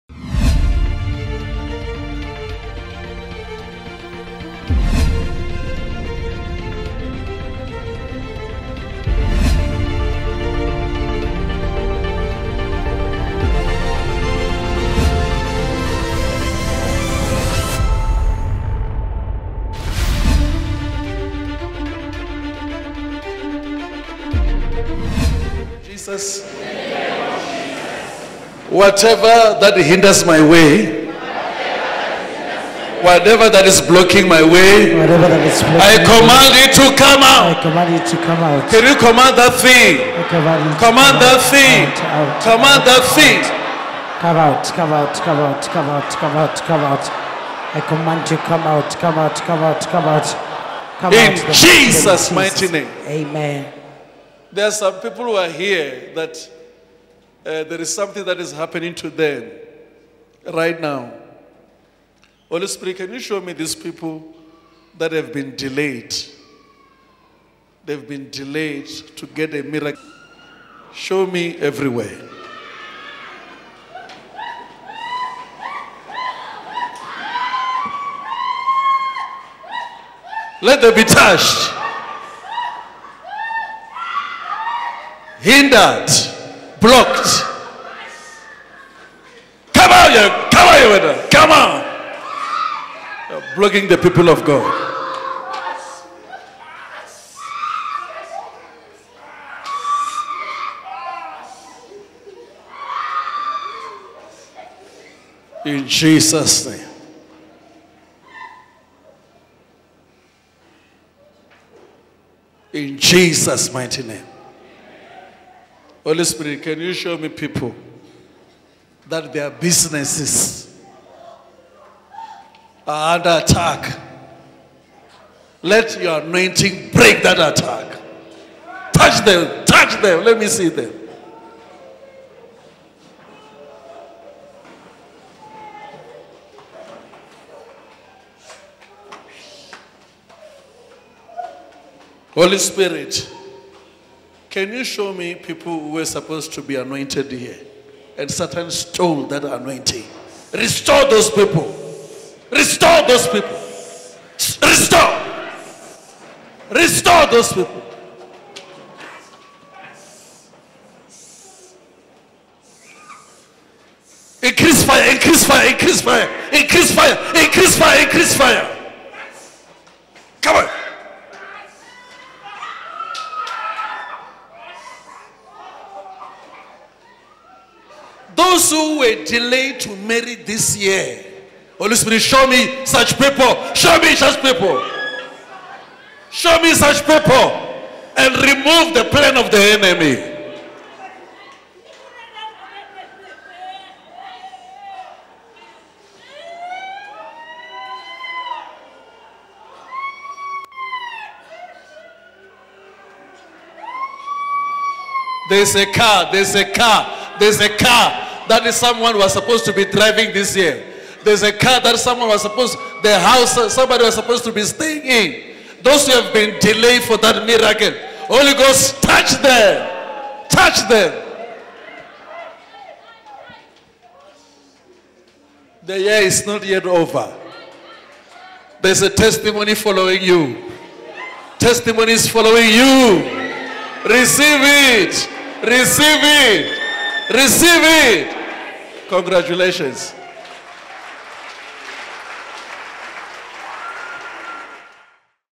HOLY SPIRIT…touch those who have been DELAYED, HINDERED & BLOCKED
Various congregants are located by the Holy Spirit and touched by the Power of God as Apostle JB Makananisa asks the Holy Spirit to reveal those delayed, hindered and blocked in various spheres of their lives